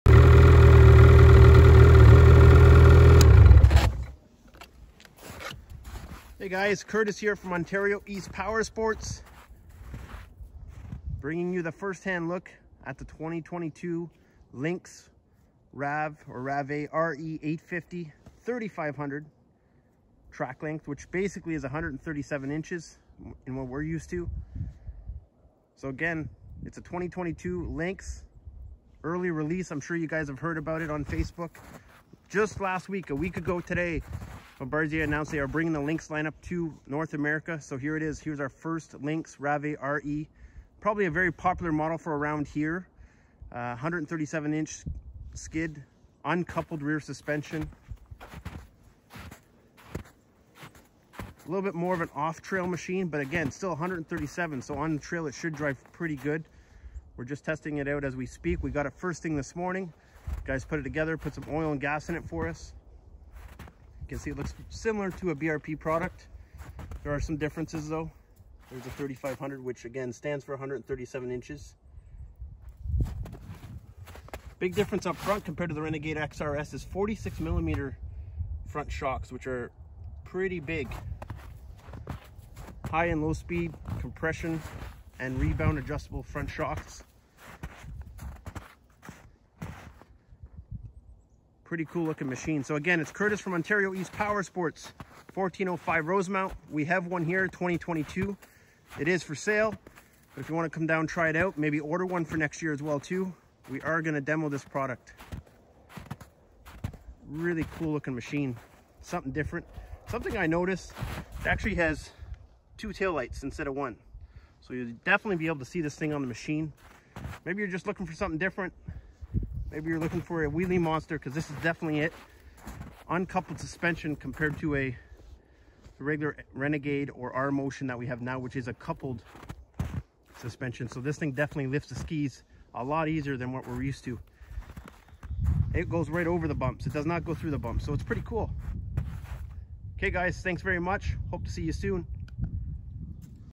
2022 Lynx RAVE 850 RE Launch - DEMO FOR SALE
Quick first impressions and specs of this 2022 Lynxx Rave RE 850! It will be available for sale this fall @ Ontario East Powersports, Cornwall Ontario. It is for sale.